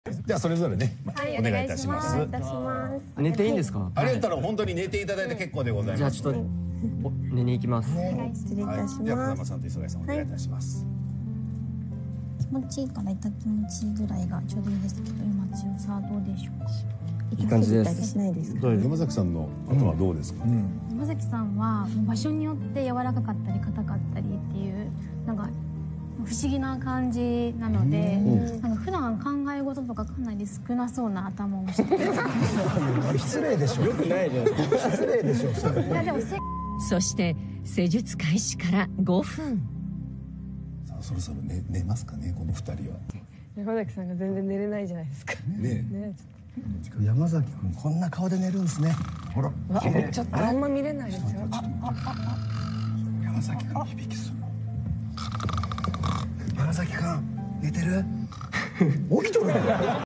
[ENG] Kento Yamazaki Getting Head Massage!
Turn on Japanese subtitles for English translation!

Source: kentooyamazaki.id on Instagram!